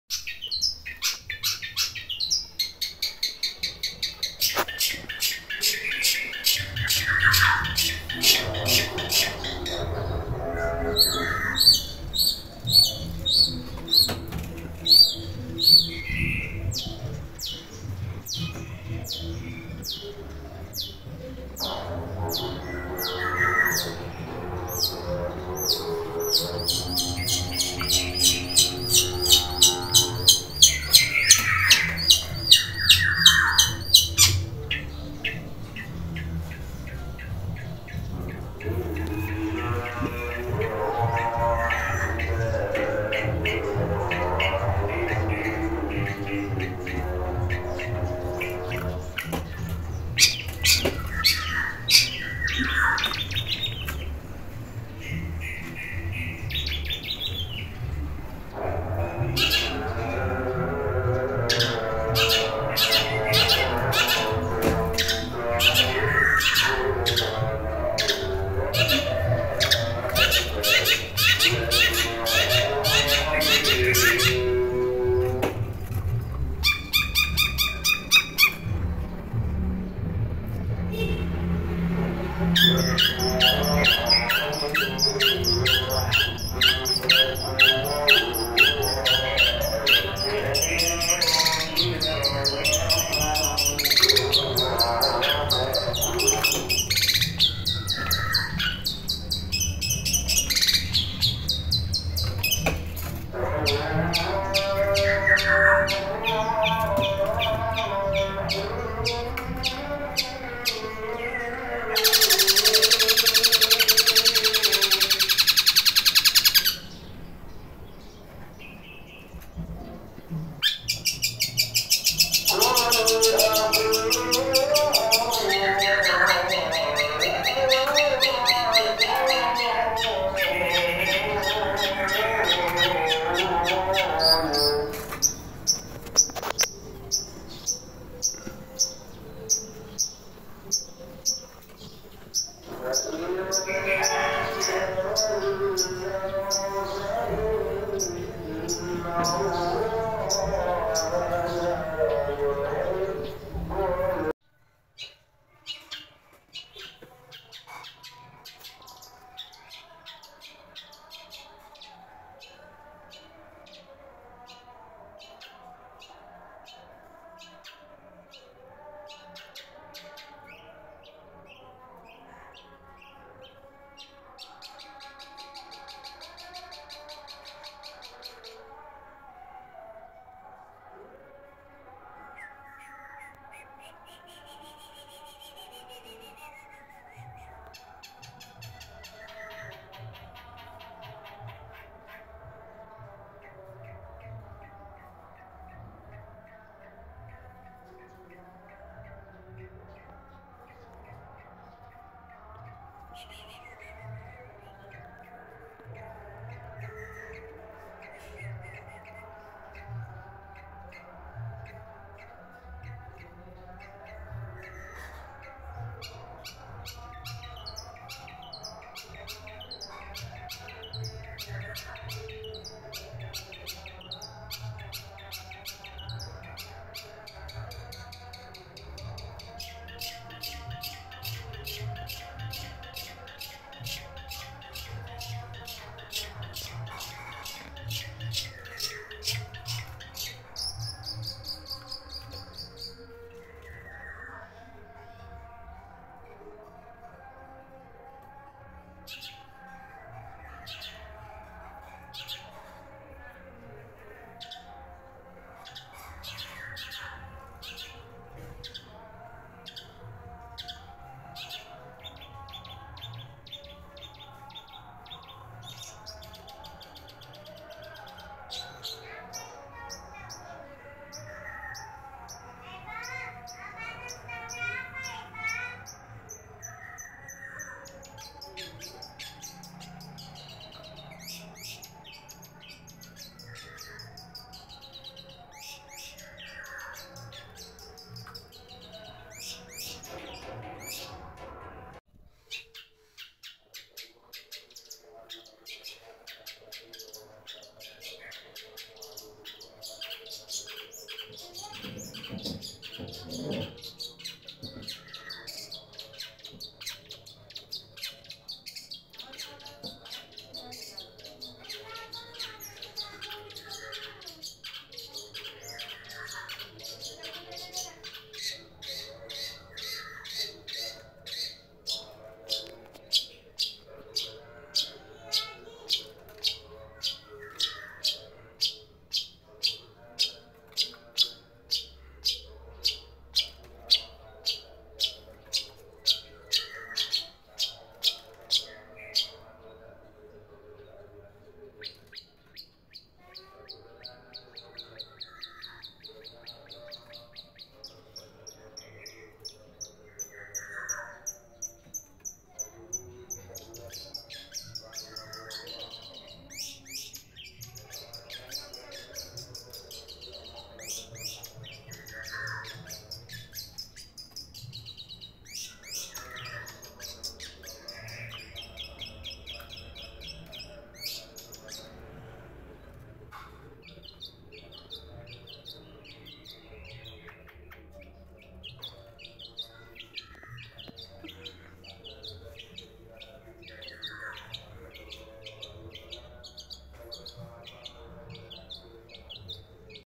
BURUNG CENDET GACOR JADI SATPAM RUMAH, ADA ORANG DATANG LANGSUNG BUNYI
Cendetnya seperti burung jalak suren, bisa jadi satpam rumah. Kalau ada orang datang langsung bunyi